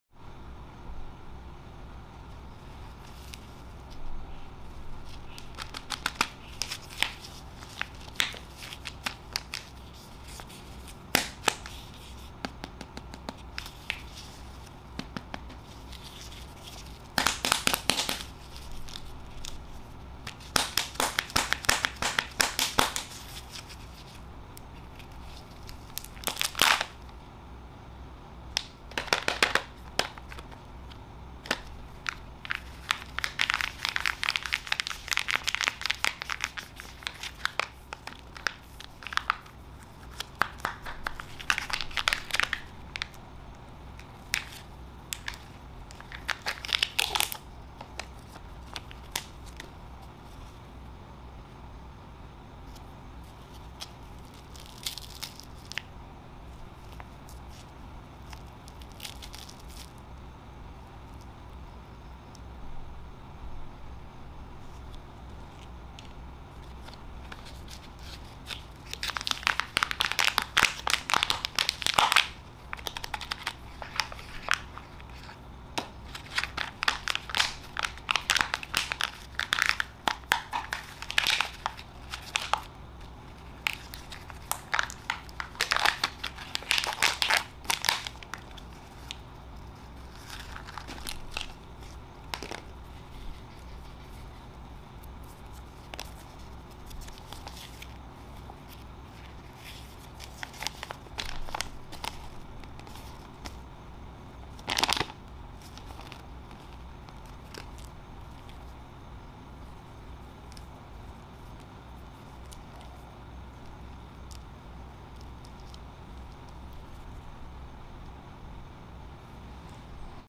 100 yummy Kinder Surprise Egg Toys Opening - A Lot Of Kinder Joy Chocolate ASMR | Sandolls
100 yummy Kinder Surprise Egg Toys Opening - A Lot Of Kinder Joy Chocolate ASMR | Sandoll

This video is not intended for children, so if you are child then close this video.